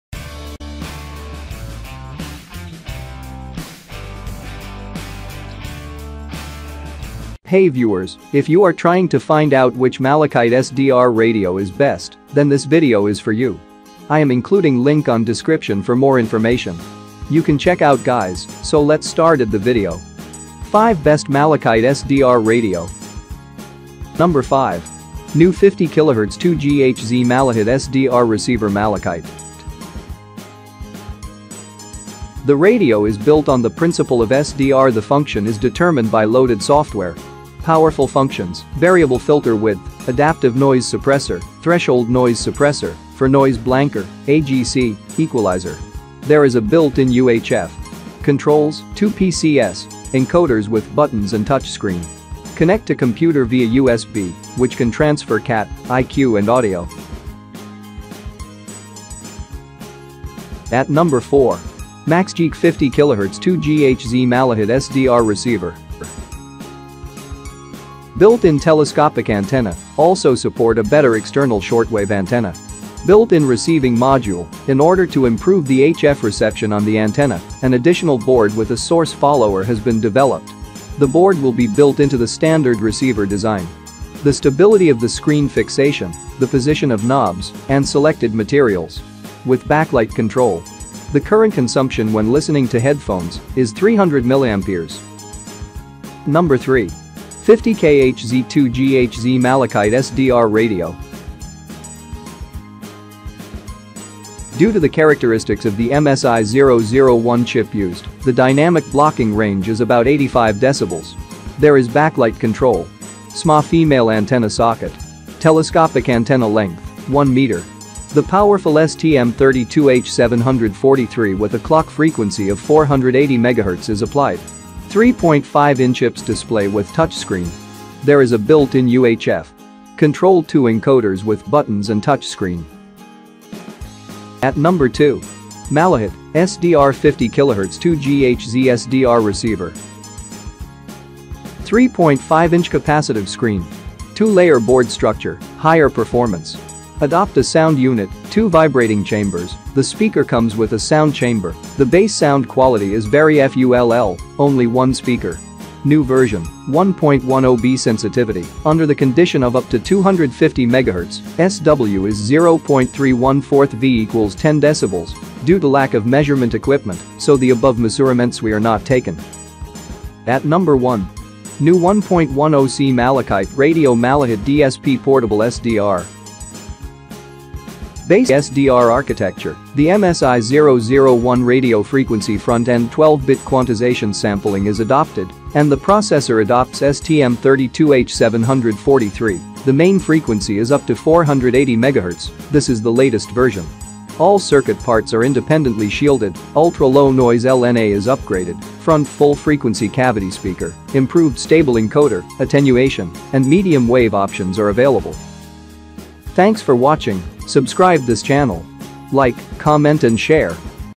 Top 5 Best Malachite SDR Radio 2022
This video is about Best Malachite SDR Radio

Here is the link of Top 5 Best Malachite SDR Radio 2022

1.Product name: New 1.10c Malachite radio Malahit DSP portable SDR

2.Product name: Malahit SDR 50KHz-2GHz SDR-Receiver

3.Product name: 50Khz-2GHz Malachite SDR Radio

4.Product name: Maxgeek 50KHz-2GHz Malahit-SDR Receiver

5.Product name: New 50KHz-2GHz Malahit SDR Receiver Malachite